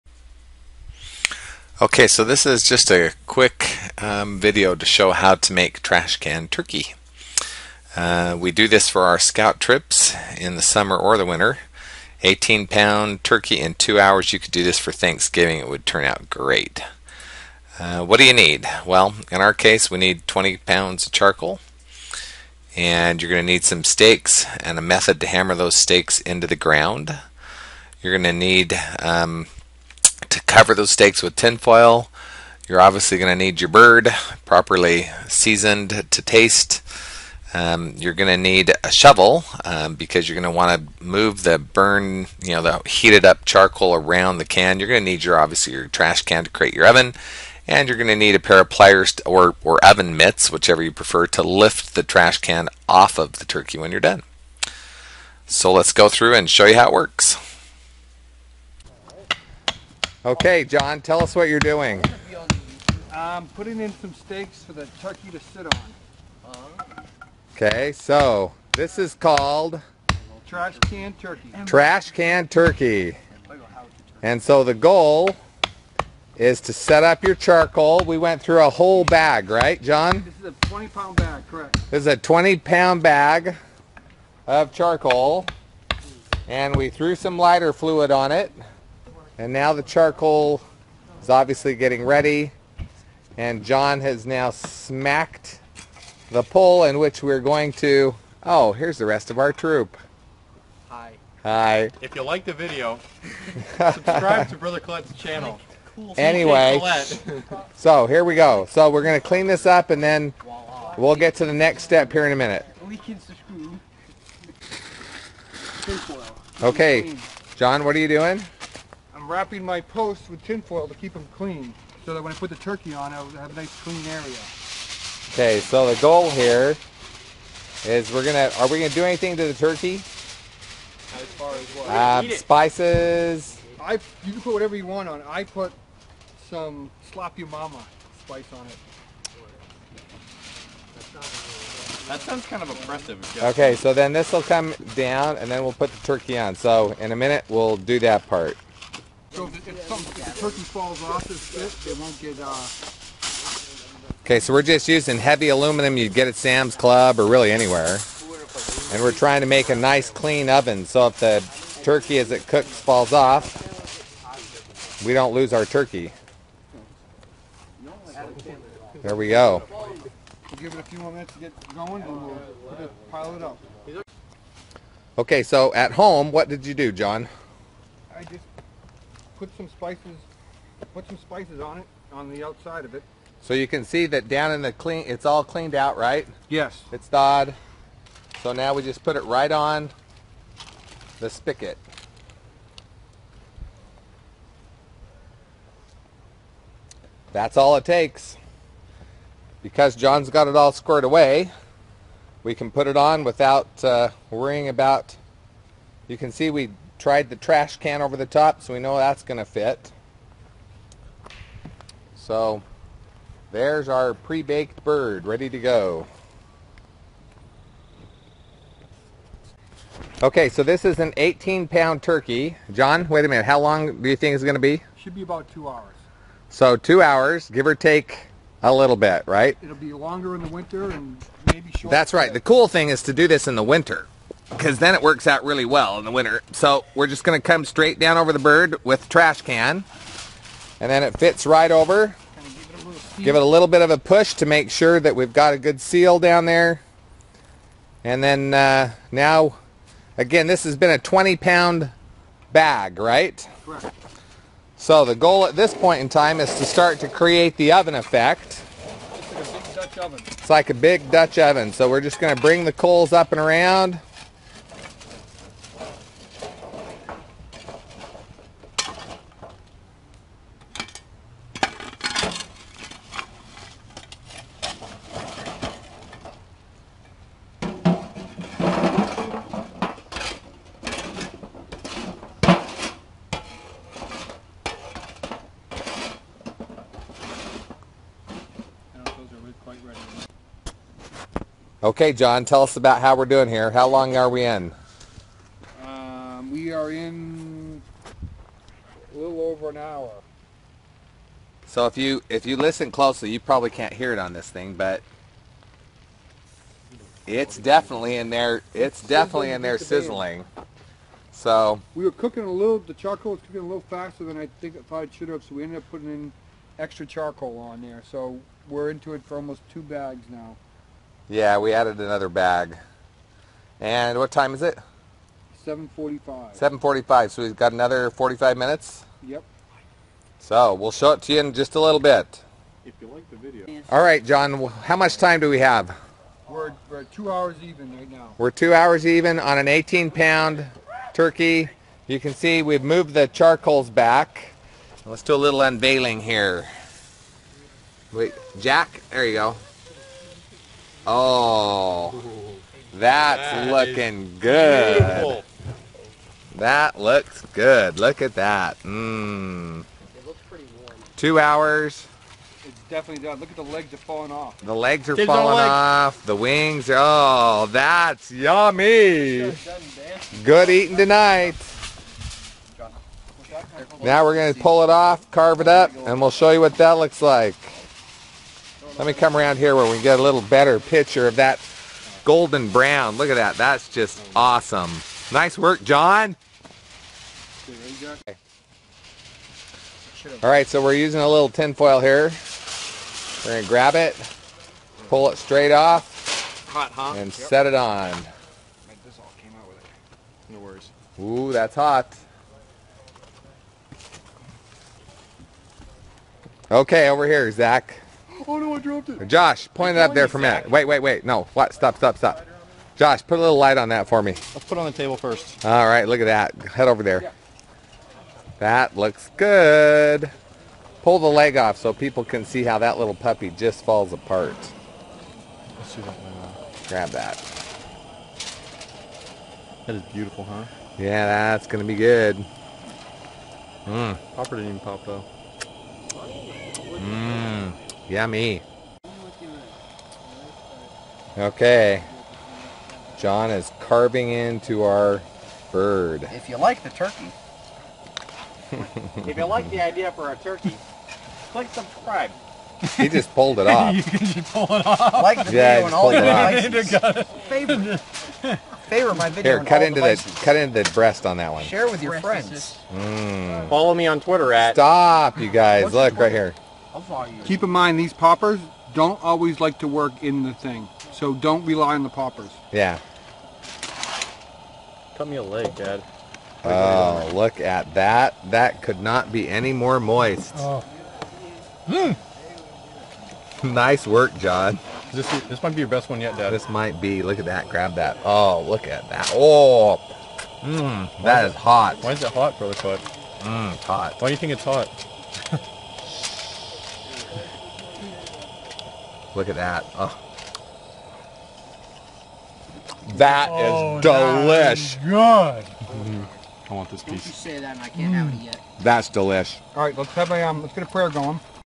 HOW TO | Cook a Trash Can Turkey
This is how our scout troop cooks an 18 pound turkey in 2 hours.  Really easy, explains what you need, and how to do it.